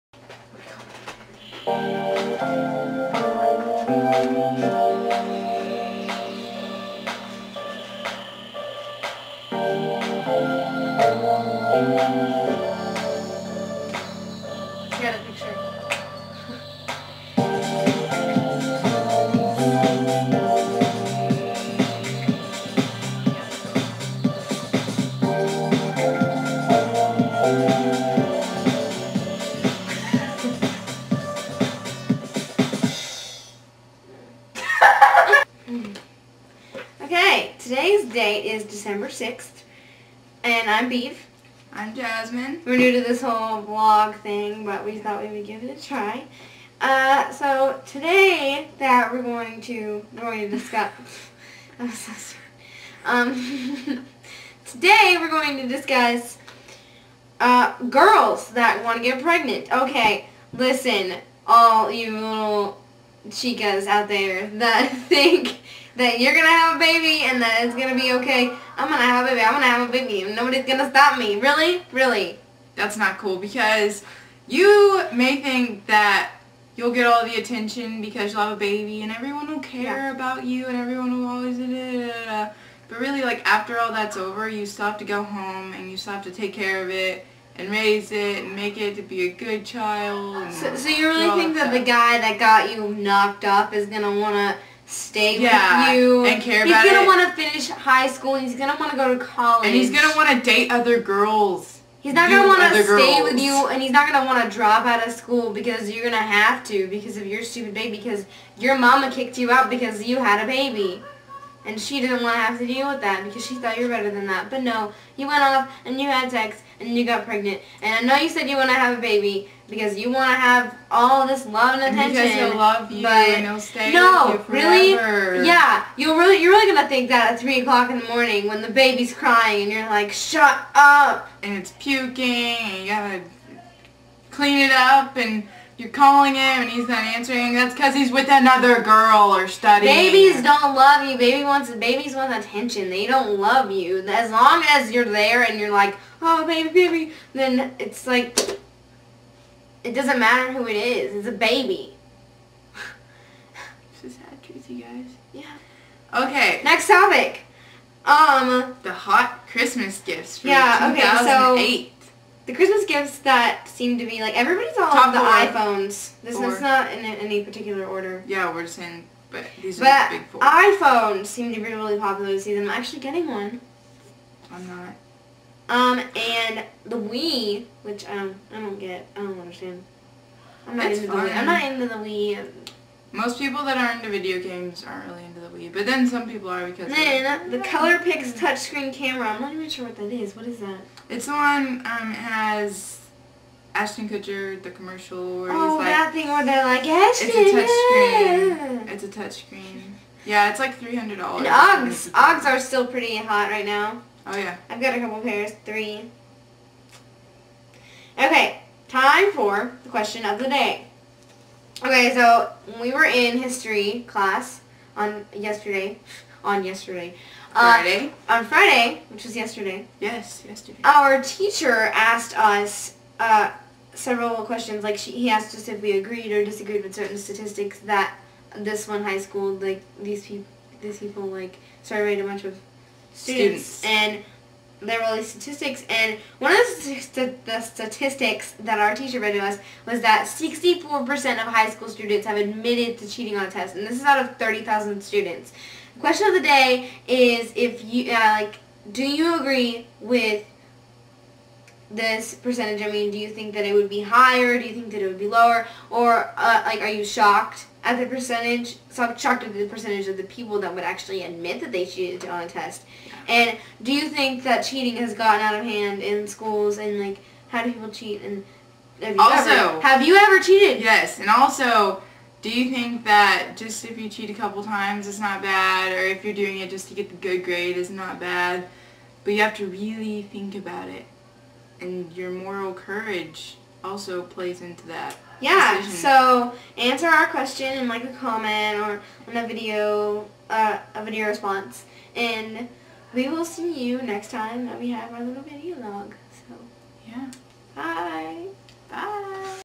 Beav and Jasmine Video Blog Numero Uno
Hiii! We're new to vlogging but this is our first vlog!!!

Discussion Issues: Pregnant Teenagers and Christmas gifts!!
Question(s) of the day: There was a survey of 30,000 students and 64% of them ADMITTED to cheating! Did you think this percentage would be higher or lower or about the same? Have you ever cheated?